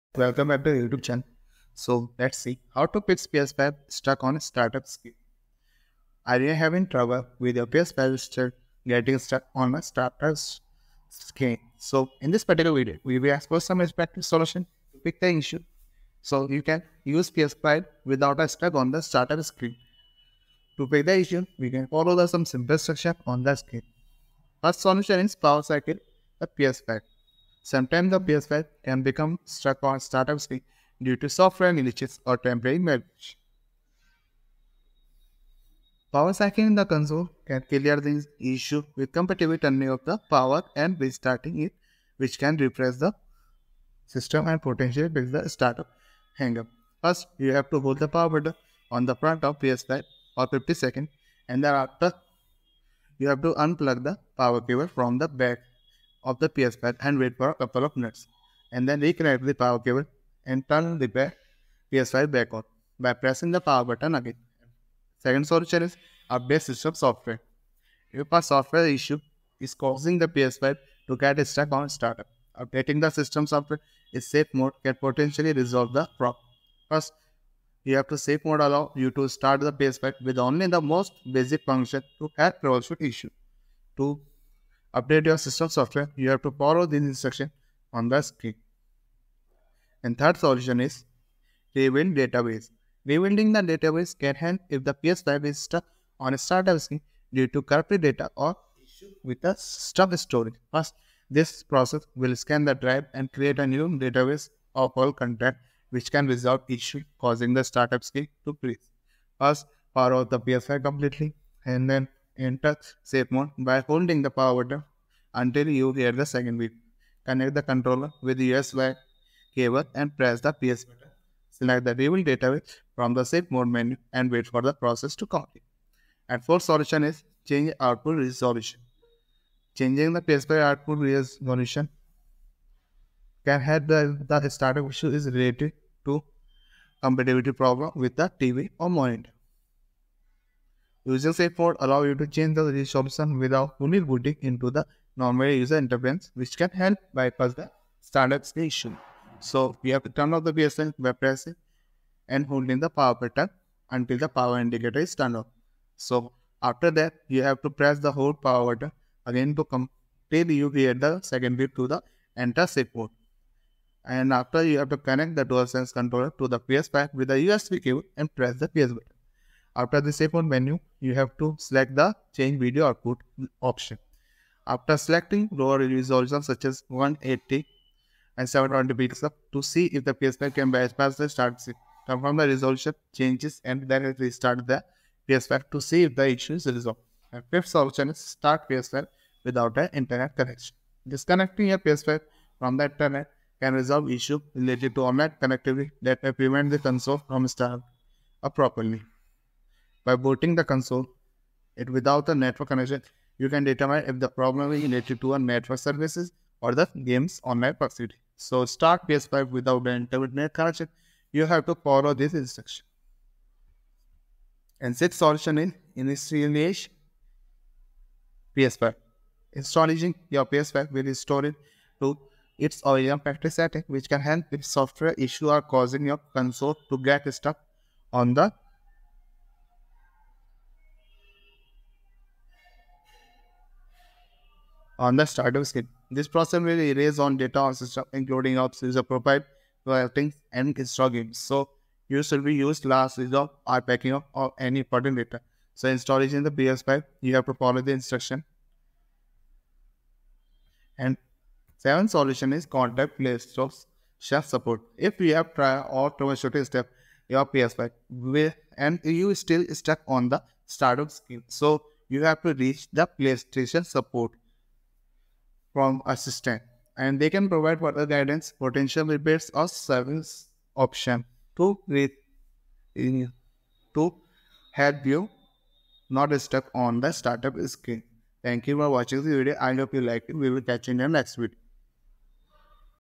How To Fix PS5 Stuck On Startup Screen
Are you having trouble with your PS5 getting stuck on the startup screen?
It can be frustrating when your gaming console doesn’t start up as expected.

00:00 Introduction
00:25 Solution 1:Power Cycle The PS5
01:06 Solution 2:Update System Software
01:31 Solution 3:Rebuild Database
02:07 Solution 4:Change Output Resolution
03:09 Solution 5:Start PS5 Without An Internet Connection
03:35 Solution 6:Initialize PS5
04:17 Solution 7:Contact PlayStation Support